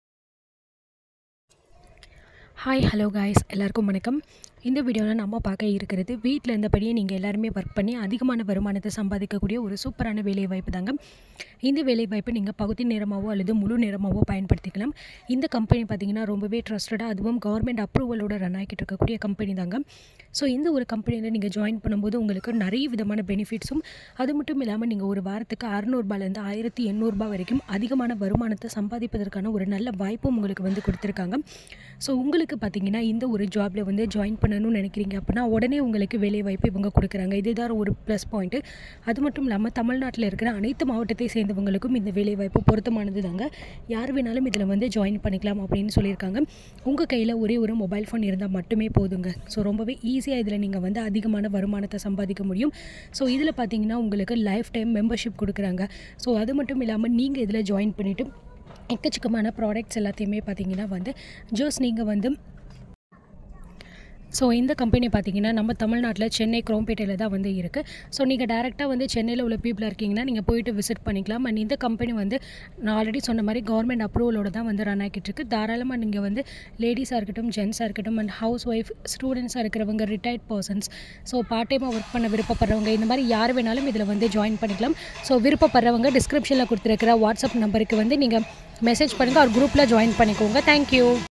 வீட்டில் இருந்து வேலை செய்து வாரம் ₹600 முதல் ₹1800 வரை சம்பாதிக்கலாம்/தமிழ்நாடு முழுதும் ஆட்கள்தேவை
⭕️வாட்ஸாப்ப் குரூப் லிங்க் :(இணைந்து கொள்ளுங்கள்)

நமது சேனல் -இல் போடப்படும் வேலைவாய்ப்பு செய்திகள் கீழ் குறிப்பிடப்பட்டுள்ளன 👇👇👇👇

📣Women Jobs
📣Mobile Jobs
📣Online Jobs

@Hiii Universe  👈👈👈